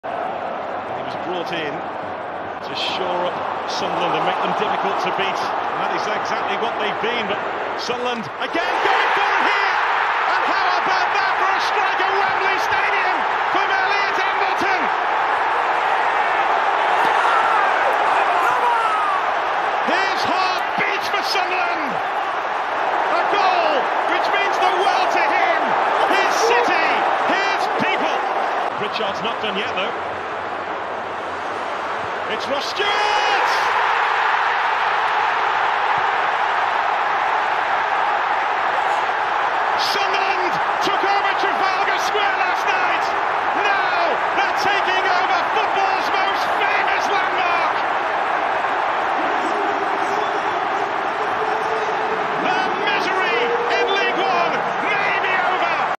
SUNDERLAND WIN 21/22 PLAY OFF FINAL
Sunderland winning the 21/22 League One Play Off Final at Wembley against Wycombe (21/05/22)

Elliot Embleton 12”

Ross Stewart 79”